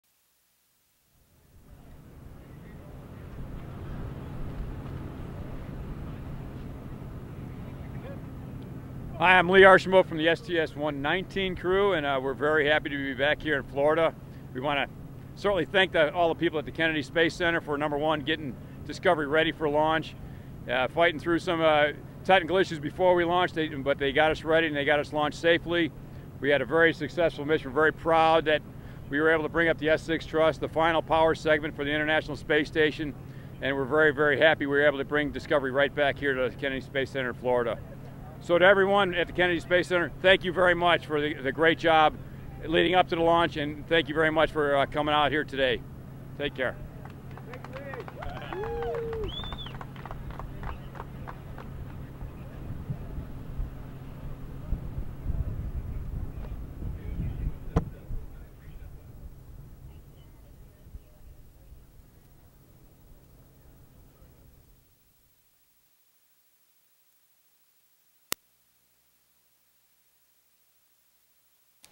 Space shuttle Discovery's commander speaks on the runway after the STS 119 landing
Astronaut Lee J. Archambault speaks after he piloted space shuttle Discovery to a landing at the Kennedy Space Center to conclude mission STS-119 which saw the installation of a final set of solar arrays for the International Space Station.